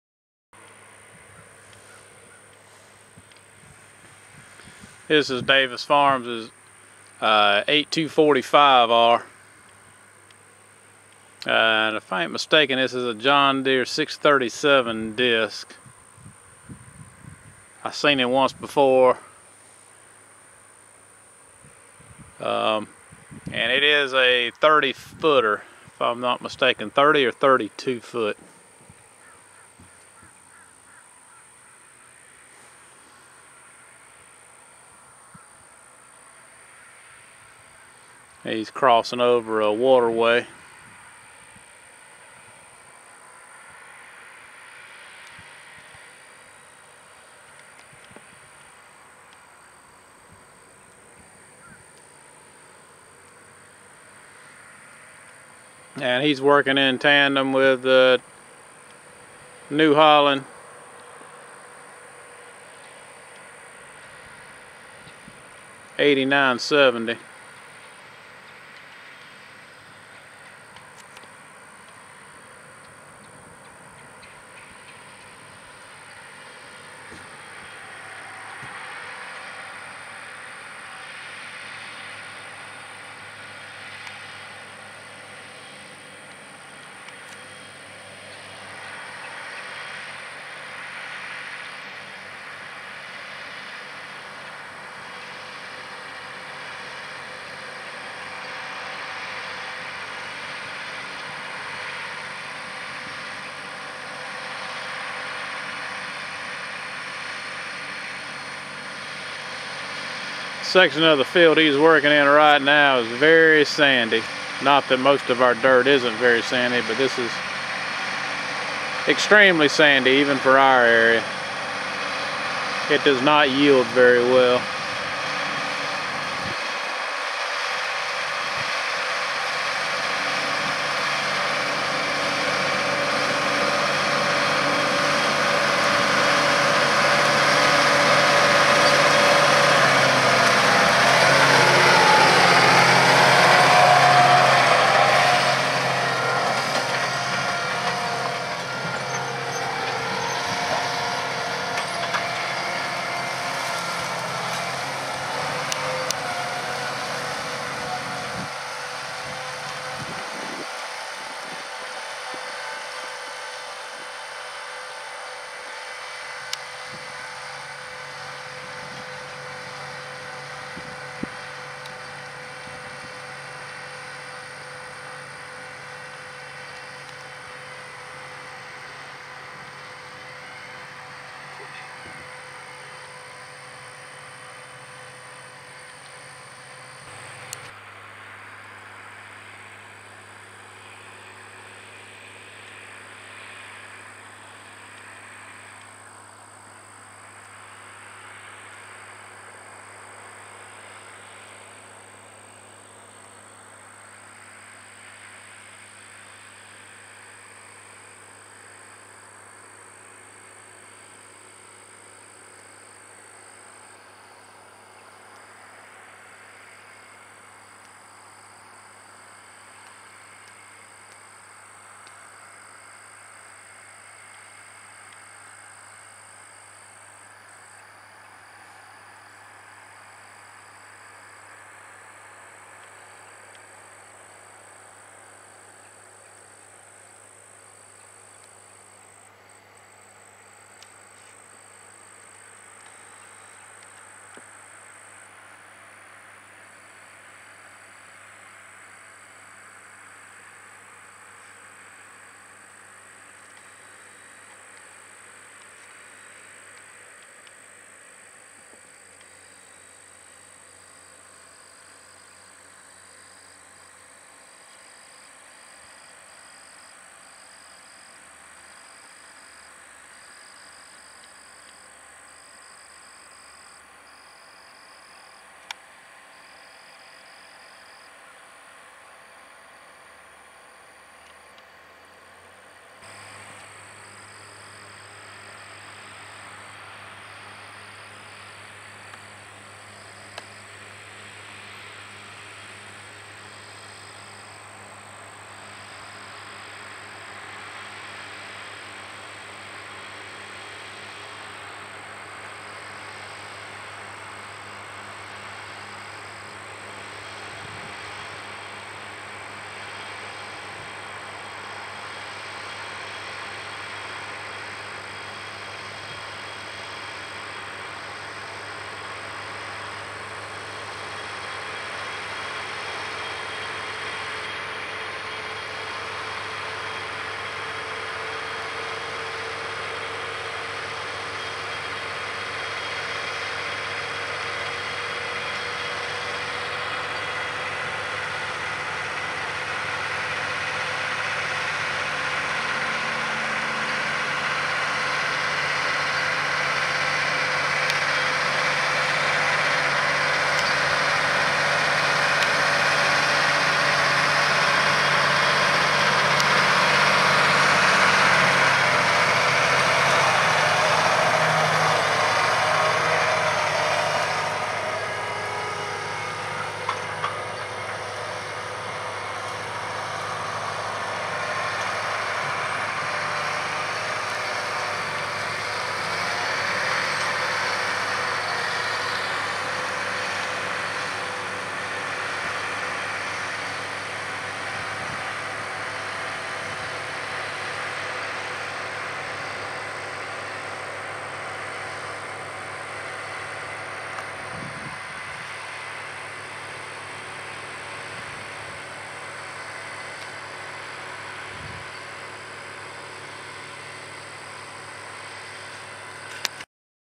8245R SPRING DISKING! DAVIS FARMS WINTER TILLAGE DISCING PT2
Davis Farms is seen in this video disking a field with a New Holland 8970 pulling a Taylor Way 580 tandem disk and a John Deere 8245R pulling a 32 foot wide John Deere 637 tandem disk. They are disking in some cotton residue in a field that will be moldboard plowed and planted into peanuts for the 2018 growing season.